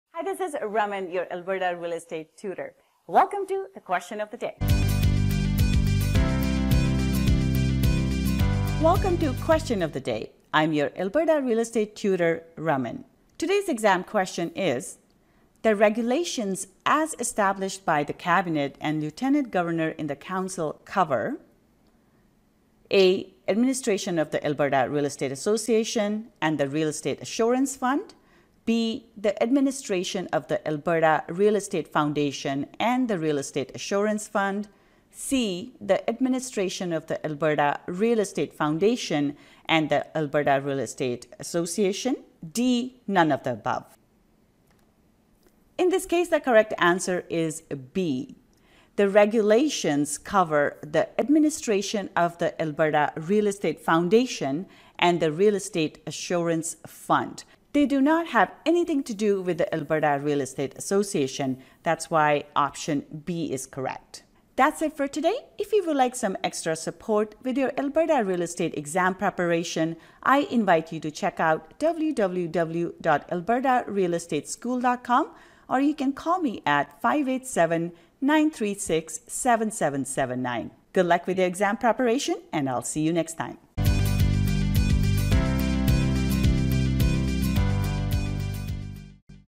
Alberta Real Estate Exam Prep Q&A: Regulations
In today's Question of the day, we look at Regulations.
Put your answer in the comments and let's see if you got it right!
Question  00:15